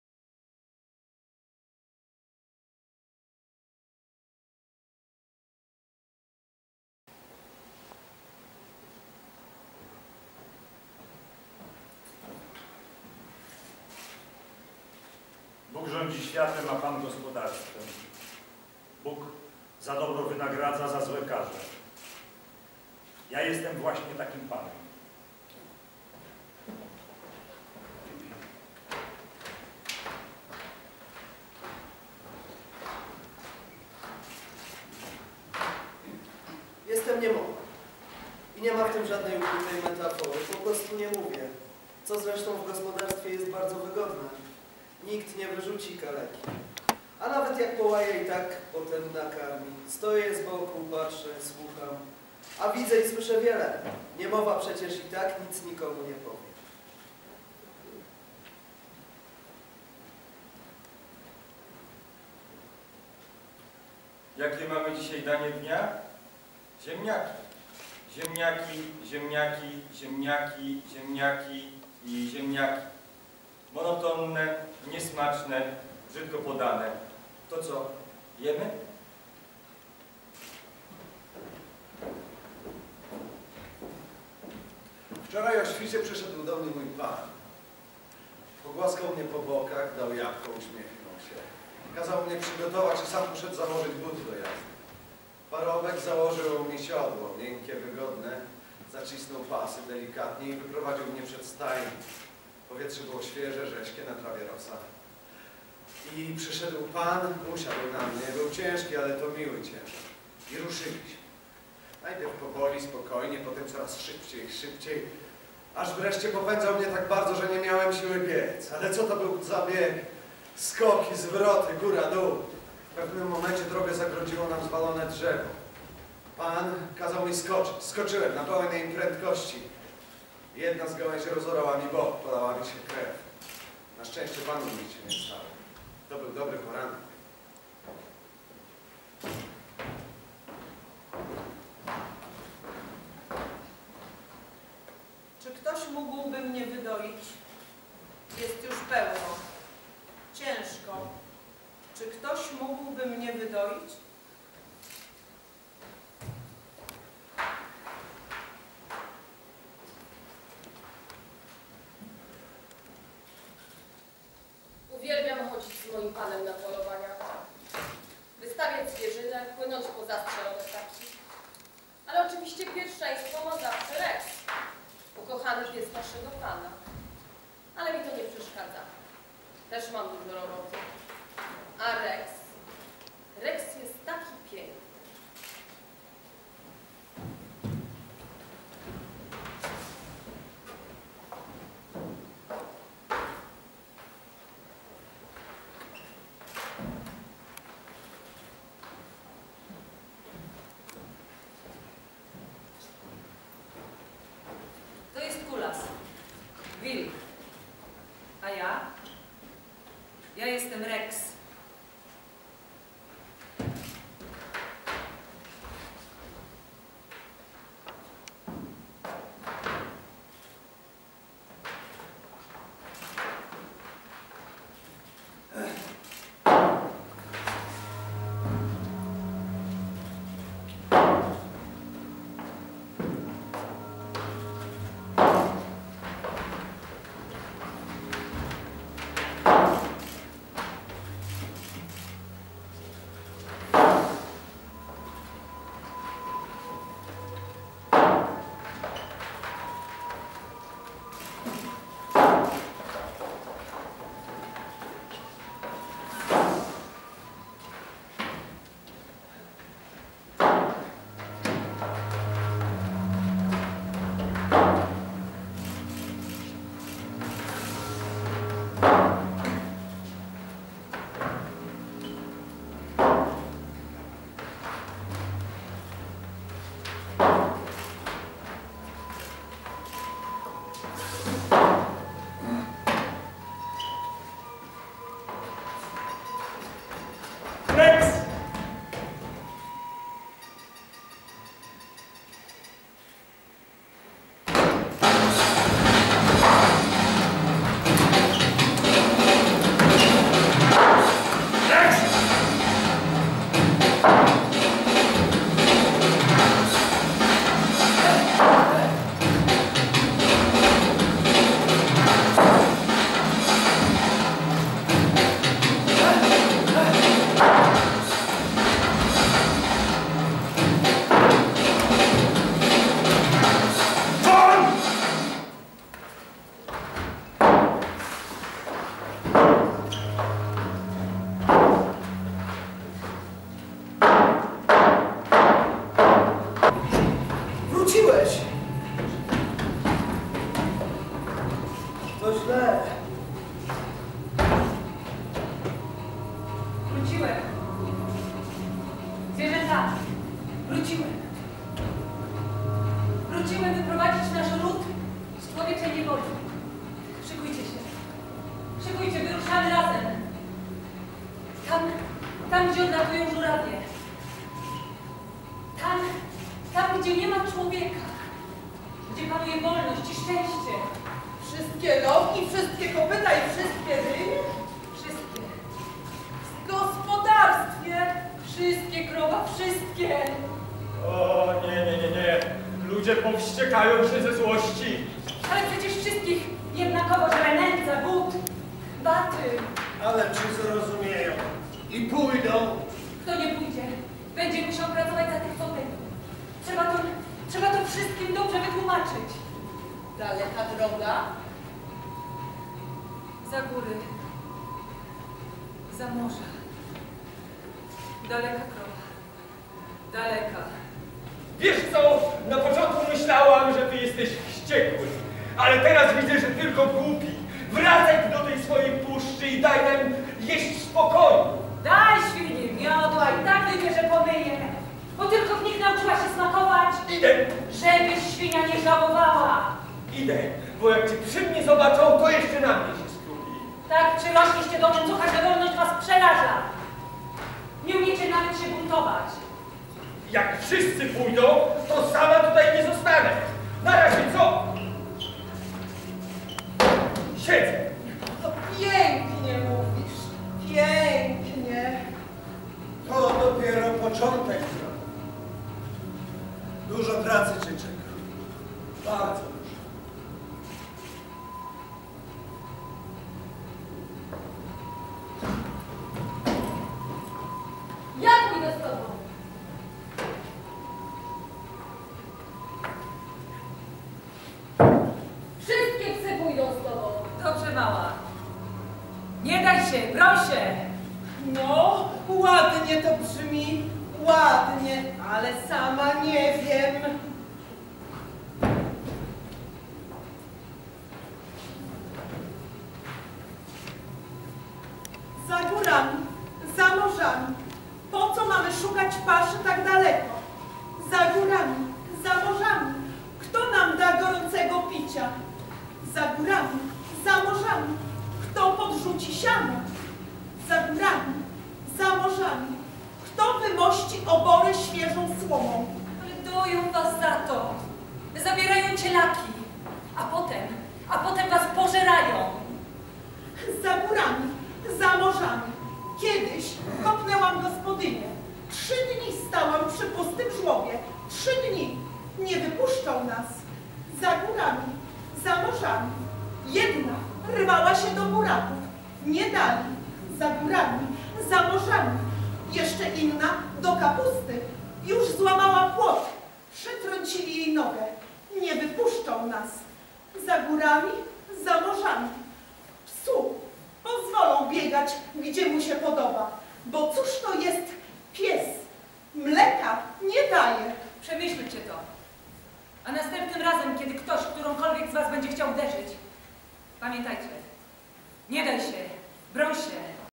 Bunt w Teatrze Miniatura: fragmenty (próba medialna)
na podstawie powieści Władysława Reymonta
adaptacja i reżyseria: Jakub Roszkowski
scenografia: Maciej Chojnacki
muzyka: Stefan Wesołowski
ruch sceniczny: Wioleta Fiuk, Patryk Gacki
obsada:
Pies Rex | Edyta Janusz-Ehrlich
Pan | Andrzej Żak
Niemowa | Krystian Wieczyński
Wilk Kulas | Jacek Majok
Wyżlica | Magdalena Żulińska
Ogier | Piotr Srebrowski
Krowa | Agnieszka Grzegorzewska
Świnia | Wojciech Stachura
dla widzów od 12 lat